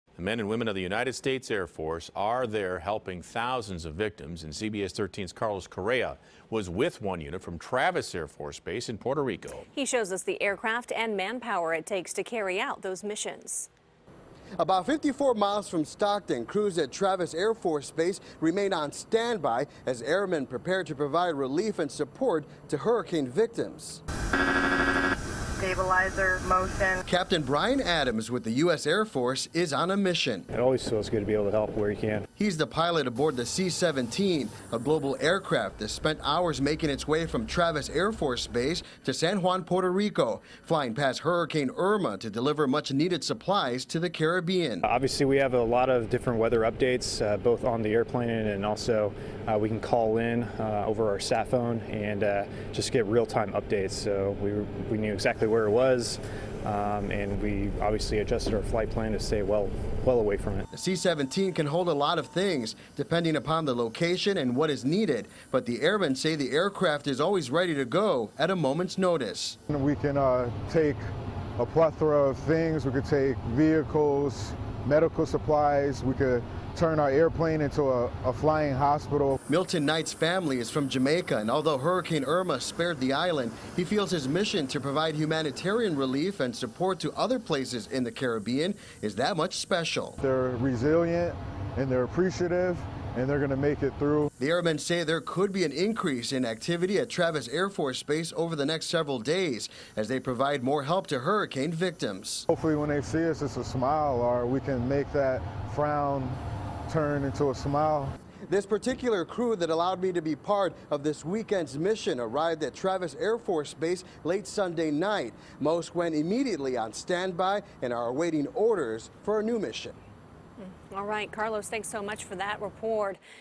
CBS13 Travels With Travis Airmen To Puerto Rico
A CBS13 crew flew with airmen from Travis AFB to Puerto Rico on a mission to help with the hurricane recovery.